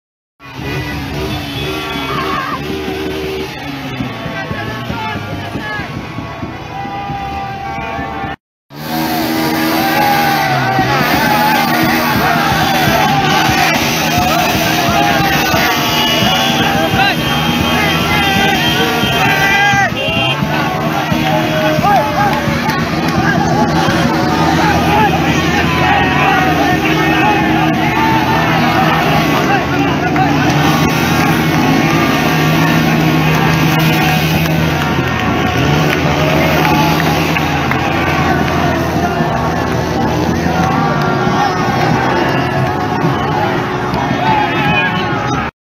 রাজশাহীর আর্জেন্টাইন ভক্তদের আনন্দ
এইরকম কিছু আর্জেন্টাইন সমর্থক বাইকার ভাইয়ারা রাজশাহীর আর্জেন্টাইন ভক্তদের আনন্দ বহুগুনে বাড়িয়ে দিয়েছিলেন।